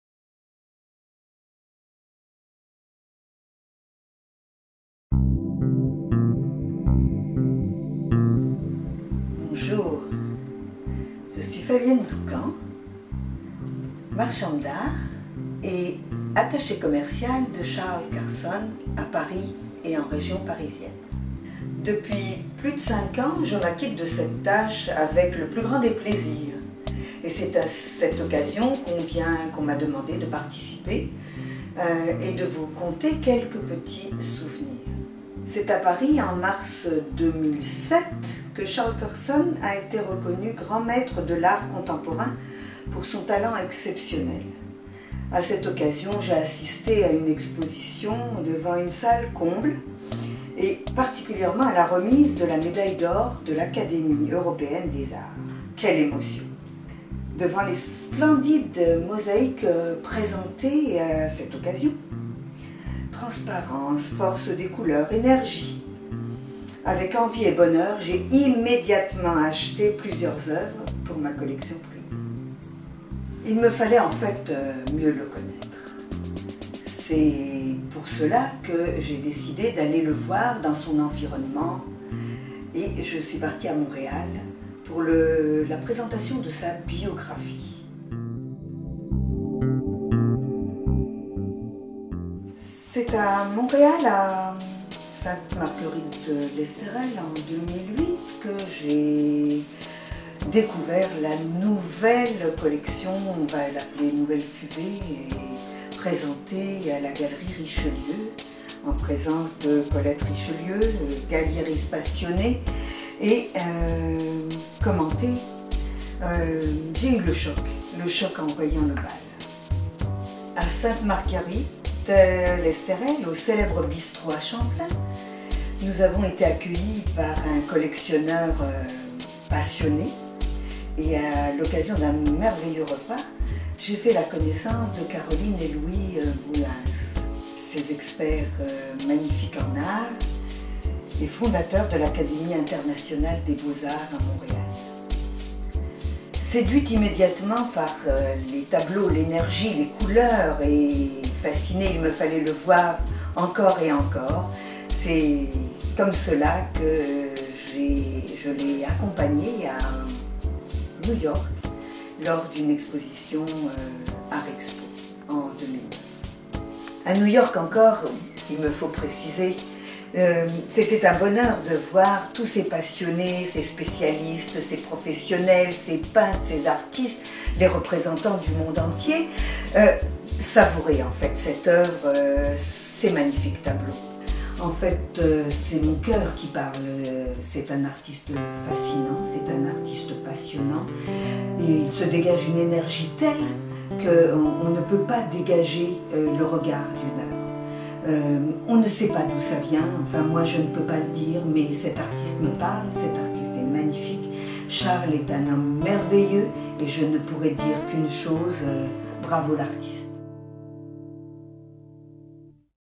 Art Contemporain - Marchand d'art DOUKHAN à PARIS - Charles Carson artiste Canadien
Now - 45 YEARS OF CREATIVITY. -  7903, rue Saint-Denis, Montréal, Québec H2R2G2

FINE ART LISS GALLERY,  - M. Brian Liss

INGLEWOOD FINE ART GALLERY - (CALGARY) - M. Michel Arseneau, director

Canadian Artist, Charles CARSON

DOCUMENTARY GRAND MASTER CHARLES CARSON
CREATOR OF "CARSONISM"

Fabienne DOUKHAN, marchande d'art, Paris.

Salon internationale des Arts France à Paris 2007

Charles Carson, médaille d'or du Salon international des Arts France, (AEAF) en reconnaissance de son talent exceptionnel.

Je pense que la renommée internationale de Charles CARSON, notre invité d'honneur de ce Salon 2007 ainsi que la qualité exceptionnelle des oeuvres exposées sur nos cimaises vous ont séduit.

Charles CARSON , créateur du courant " le Carsonisme est un novateur. Le fondateur de l'Académie internationale des Beaux Arts du Québec : Louis BRUENS , historien d'Art, décrit très bien son art "Il s'agit d'une peinture distincte de tout ce qui s'est fait, et de tout ce qui se fait à notre époque depuis longtemps".

Lauréat en 2002 de «La grande Médaille d'or de Rayonnement Universel» la plus haute distinction du Salon international des Beaux-arts de Montréal "en reconnaissance de son talent exceptionnel et pour marquer le couronnement de son œuvre" et en 2006 Médaille d'OR de de l'Académie internationale des Beaux Arts du Québec.  Mention d'honneur lors de la neuvième édition des MIM d'or 2003, du Salon des technologies au Palais des Congrès de Montréal;

Mot de la présidente : Madame  Marie LAURÈS

Comment je ressens sa peinture ? Elle présente deux facettes : d'une part, son amour de la nature sublimée, tantôt évoquant l'embrasement d'un coucher de soleil, tantôt évoquant des instants de la vie animale et végétale, d'autre part traduisant dans ses oeuvres mosaïques. son paysage intérieur.

Sa peinture tonique, onirique, dans laquelle des formes vivantes se déplacent à travers d'autres formes, réelles ou inventées, un entrelac de couleurs baignant dans des superpositions habiles, et des transparences subtiles, le tout exécuté en touches obliques accentuant l'effet de mouvement et de scintillement lumineux.

J'ai le plaisir de décerner à Charles CARSON la médaille d'OR, au titre de Grand Maître académicien, de L'Académie Européenne des Arts-France pour la qualité exceptionnelle des oeuvres présentées au Salon international des arts france 2007 de l'Académie Européenne de AEAF.